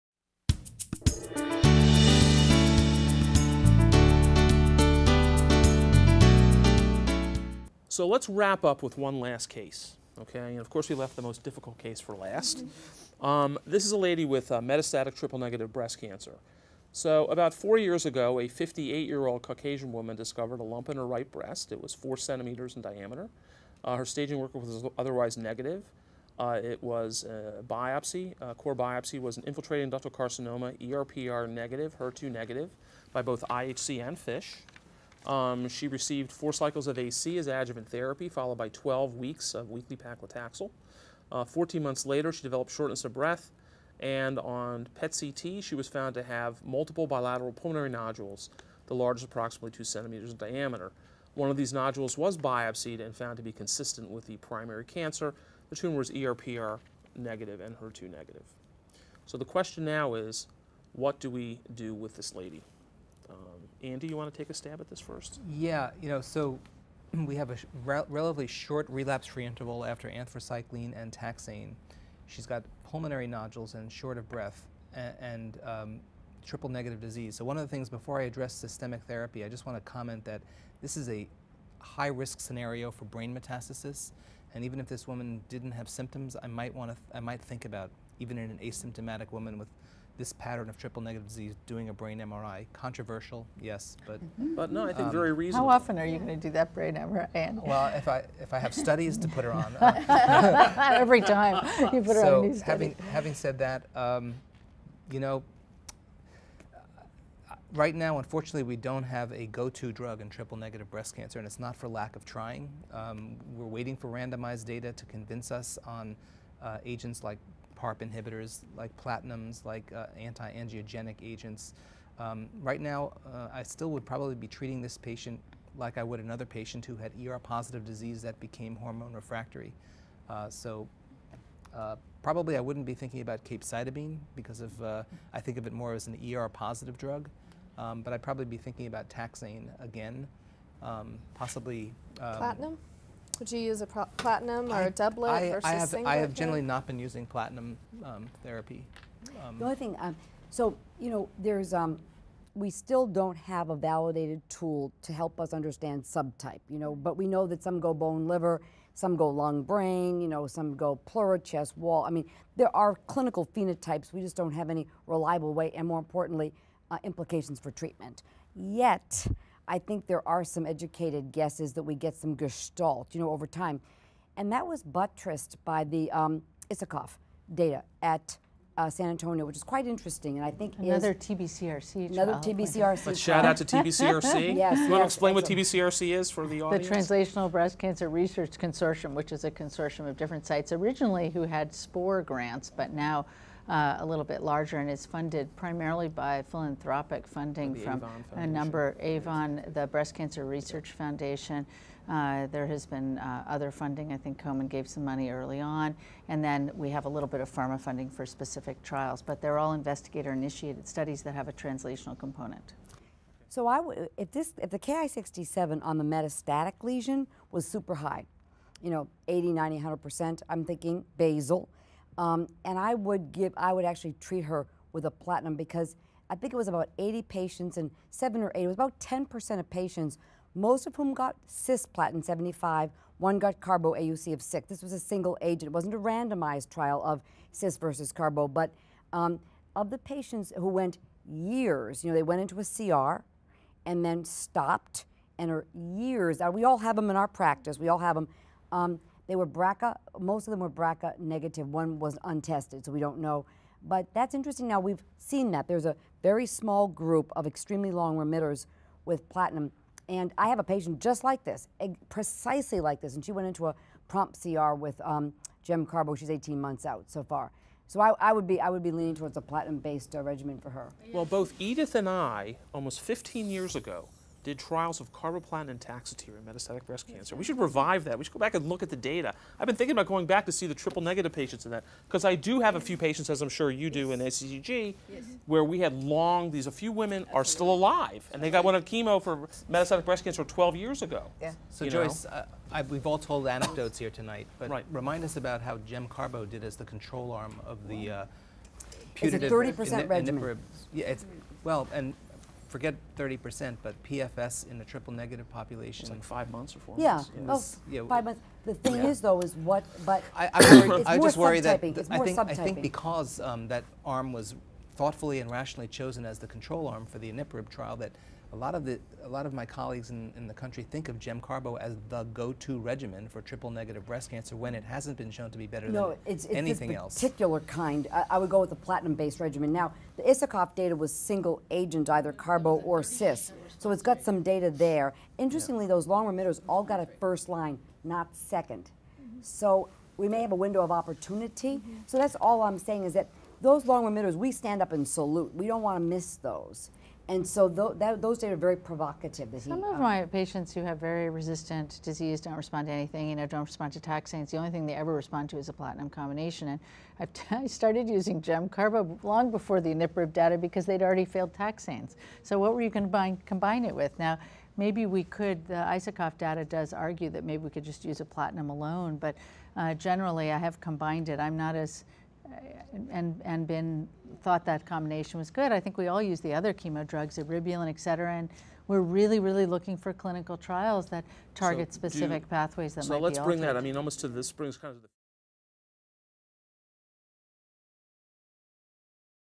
Case Study: Treating Metastatic Triple-Negative Breast Cancer
In the final case study of the discussion, panelists describe the treatment of a 58 year-old Caucasian woman with metastatic triple-negative breast cancer.